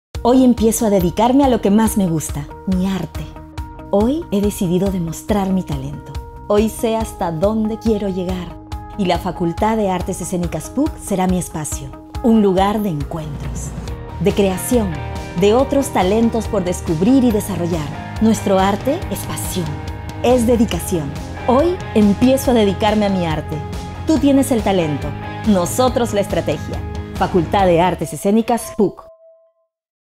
Estudia especialidades artísticas en la PUCP. - ¡Sé un profesional del arte!
La PUCP ofrece 4 carreras para profesionalizar tu pasión en las artes escénicas: Música, Danza, Teatro, y Creación y Producción Escénica.